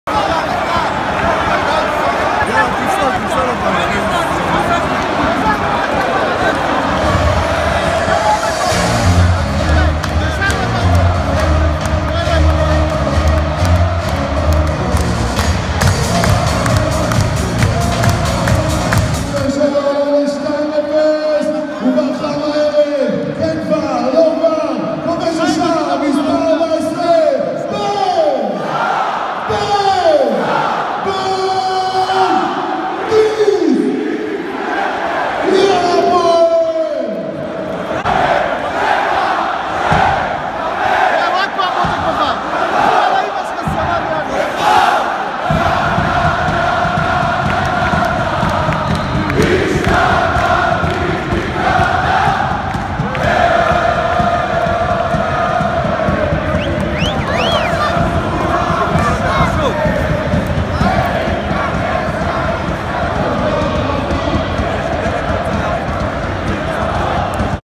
הפועל באר שבע , הכרזת השער השני - מחזור 23 2019/20 ( צילום A.T)
הפועל באר שבע , הכרזת השער השני - מחזור 23 2019/20
במשחק בו ניצחה את נס. ציונה בתוצאה 0:3

צילום A.T -Amirav Tzur